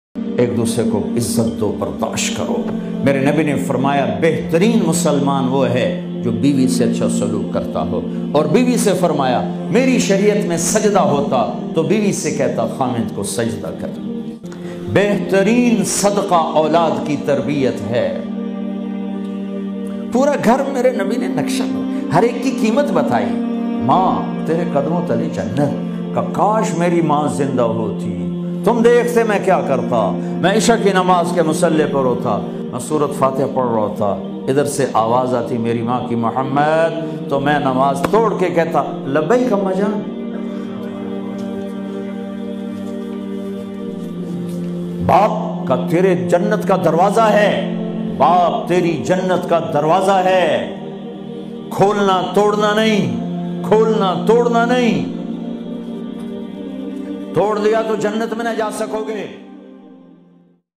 MAAN BAAP MIAN BIVI Ki Ahmiyat ISLAM Mein | Khoobsurat Bayan By Maulana Tariq Jameel Sab WA Status
Islam Mein MAAN, BAAP, SHOHAR Aur BIVI K Darjaat Or Unki Kia Ahmiyat Hai by Maulana Tariq Jameel Sahab, Khoobsurat Bayan.

If You Like This Video Please Click on The Like Button, Share It With Your Loved Once,

Thanks 😊